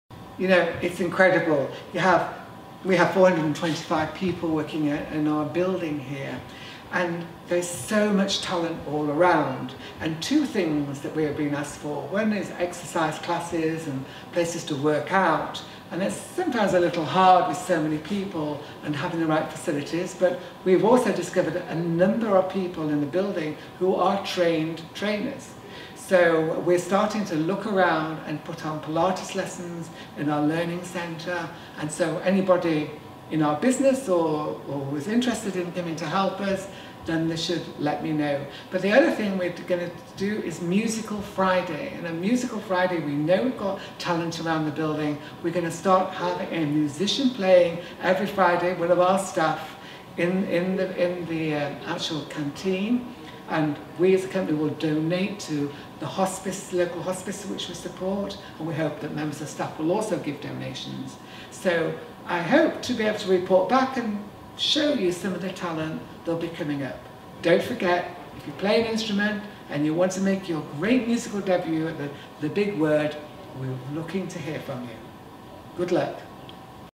Music Fridays at thebigword
There is so much talent in our company.

We have been asked for exercise classes and places to work out. It is sometimes a little hard but we have discovered that there are a number of trained trainers here. So we are looking at starting Pilate classes in our Learning Centre. If anybody in our business is interested in helping out, let me know.

We're also launching musical Fridays at thebigword. We are going to start having musicians playing and the company will donate to our local hospice and I hope staff will also be able to donate.

If you play an instrument and want to make your great musical debut at thebigword, we are looking to hear from you.

Good luck!

Larry